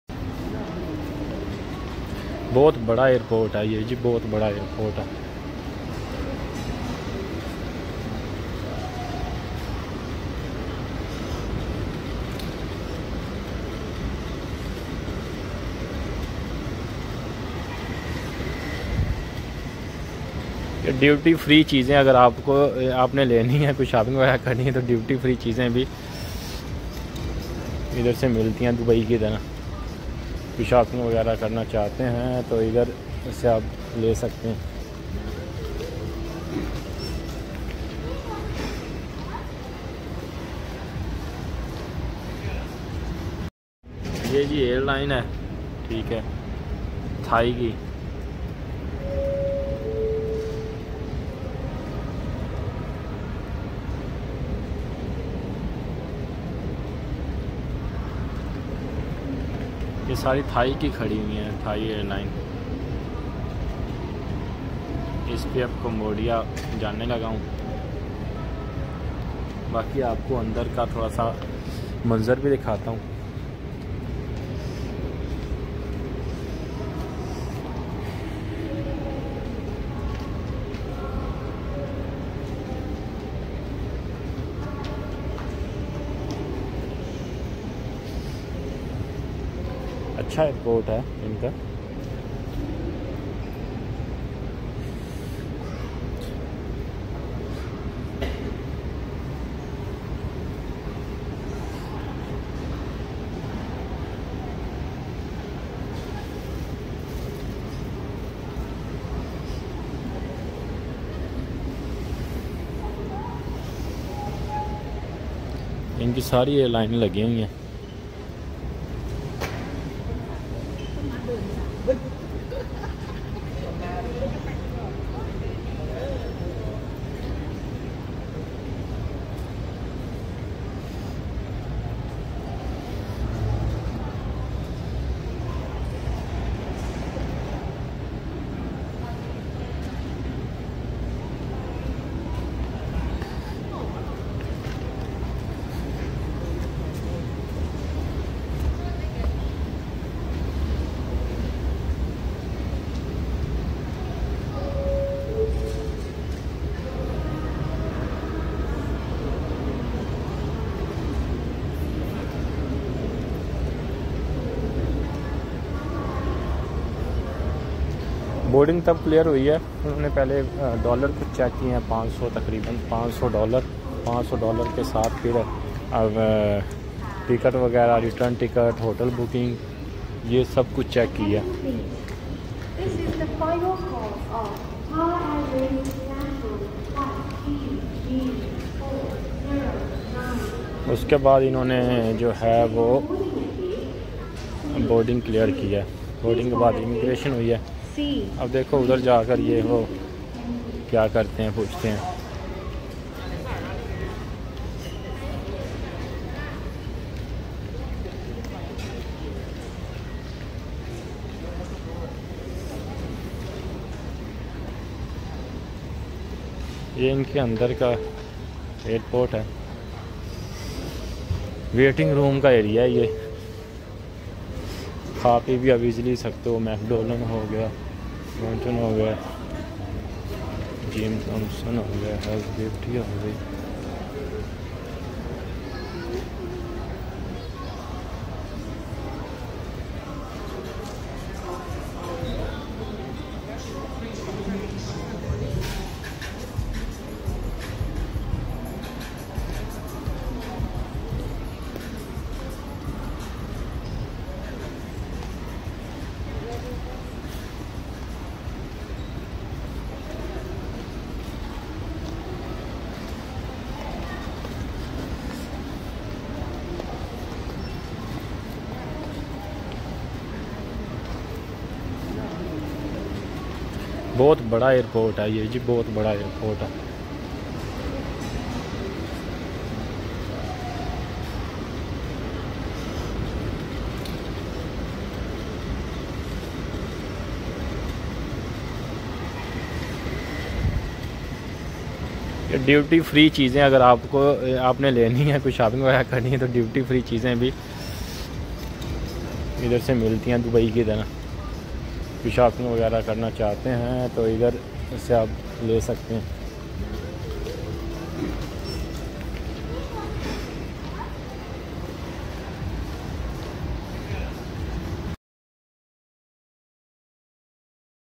BANGKOK SUVARNABHUMI INTERNATIONAL AIRPORT WALKING TOUR(HINDI)PART 13! SECRETS OF BANGKOK AIRPORT!
Hi

About this video:

In this video, BANGKOK SUVARNABHUMI INTERNATIONAL AIRPORT WALKING TOUR(HINDI)PART 13! SECRETS OF BANGKOK AIRPORT!

I will explain how Bangkok airport's an inside view and how you can face immigration issues. All secrets of Bangkok airport are here.

Queries Solved:
HOW to solve immigration issues at the airport.
How Bangkok airport inside.
How is Bangkok  airport vlog